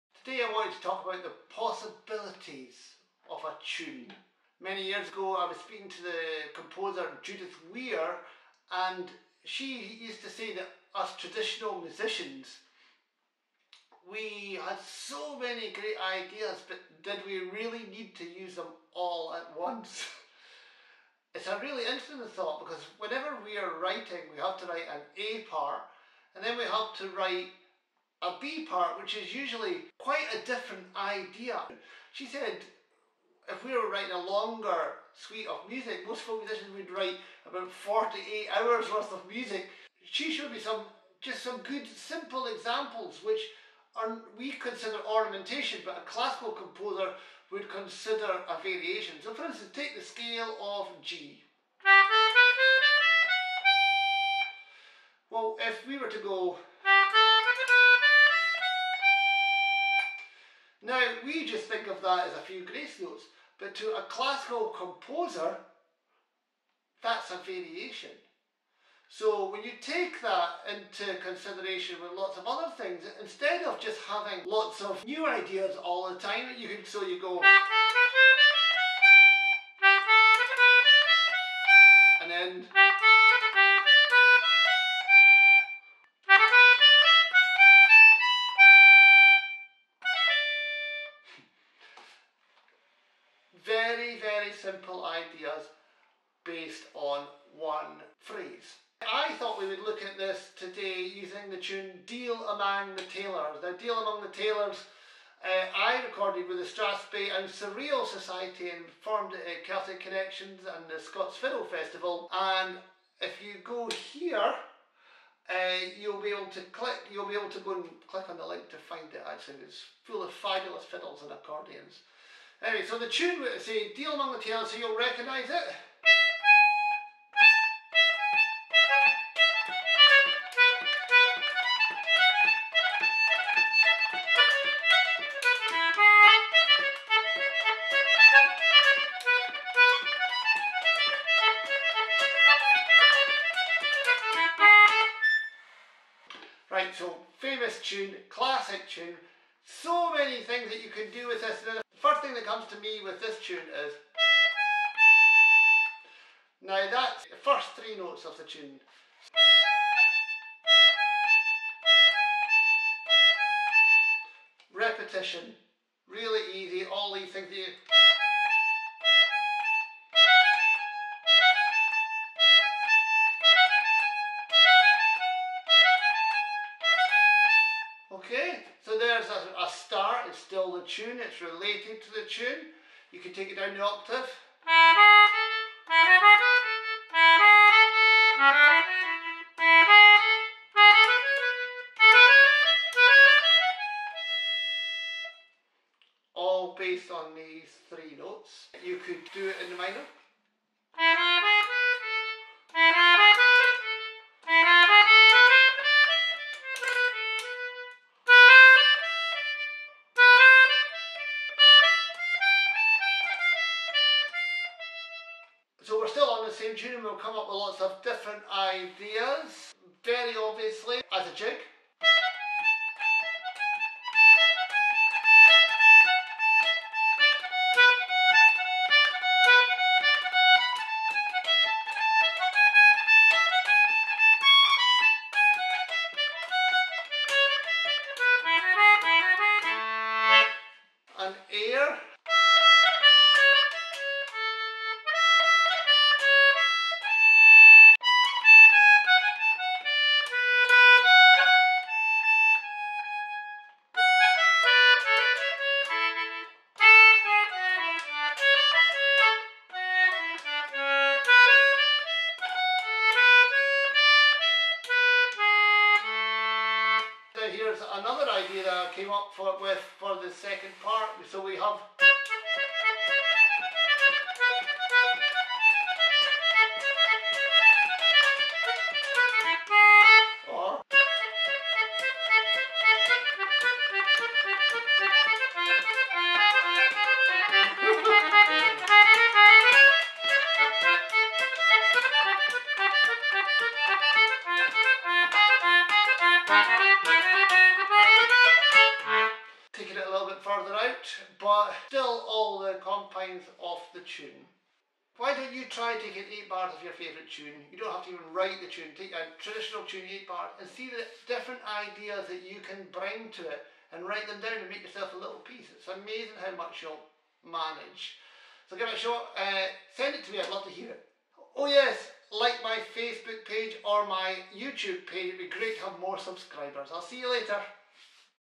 Simon Thoumire vlog 9 - Composition ideas for developing themes for a Scottish reel
If you are asked to composed a longer piece of music what do you do? Do you write lots of tunes and stick them together or do you take a phrase and expand on the theme? Here’s some ideas for the latter!